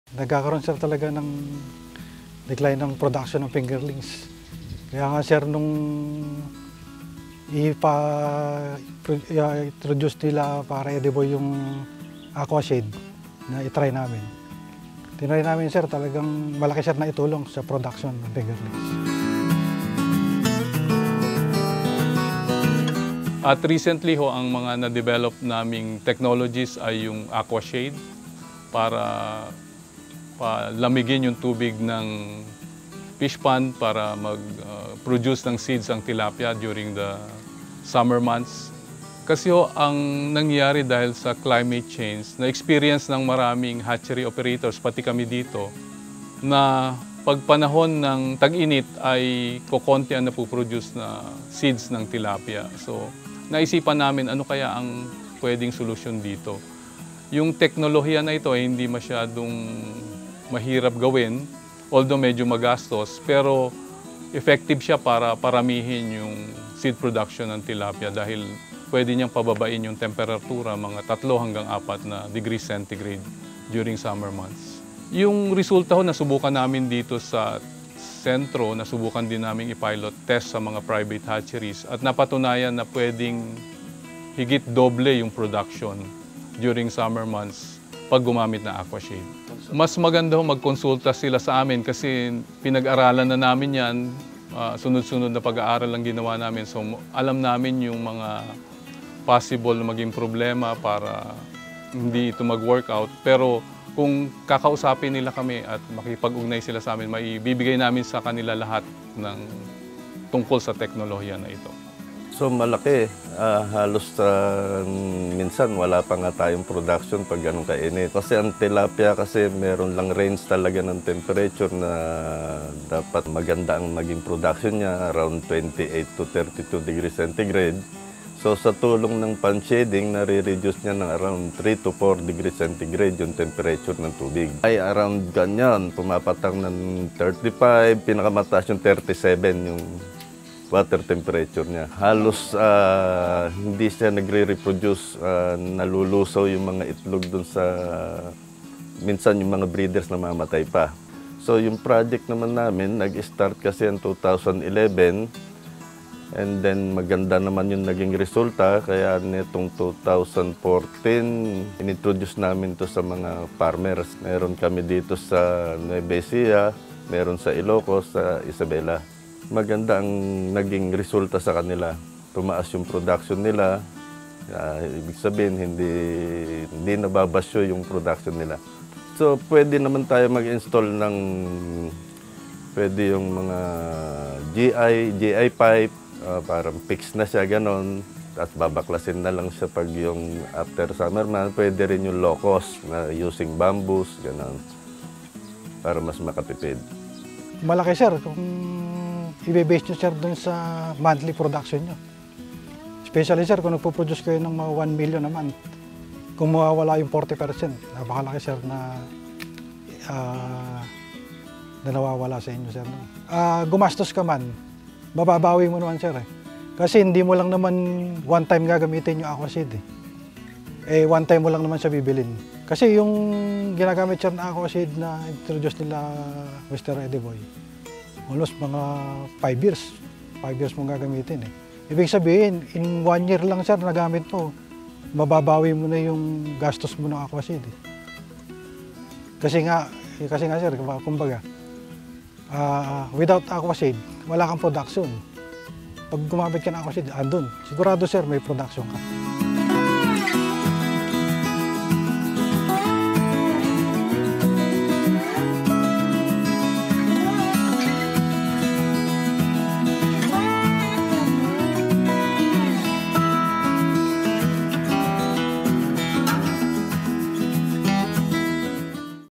Tilapia Farming: Increasing Fingerling Production using Aquashade
Tilapia farming - increasing fingerling production using aquashade technolog.

Tilapia farming in the Philippines during summer,  production of fingerlings decreases due to high temperature, but with the use of aquashade, farmers can continue to produce fingerling with high percentage of production.

Aquashade was develop by Fresh Water Aquaculture Center of Central Luzon State University.

Agribusiness How It Works. Instruct. Inspire. Succeed.
Agribusiness in the Philippines, opportunities for the OFW and their families.